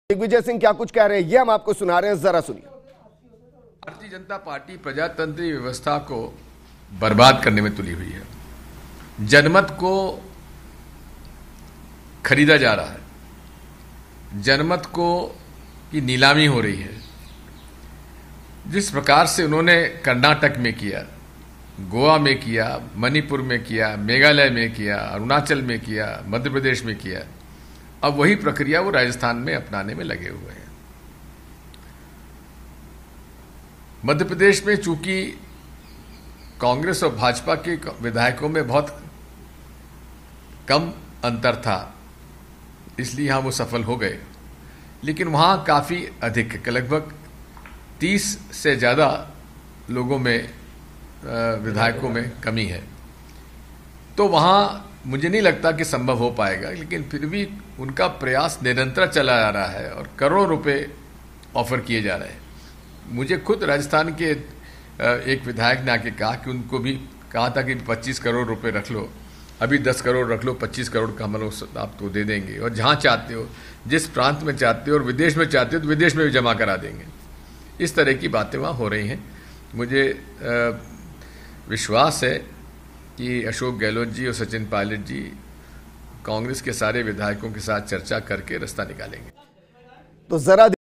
मुझे विश्वास है Gehlot और Pilot बैठकर सुलह करेंगे: Digvijaya Singh | Rajasthan Political Crisis
About this Video:
I believe Gehlot and Pilot will sit down and reconcile: Digvijaya Singh | Rajasthan Political Crisis
About Channel: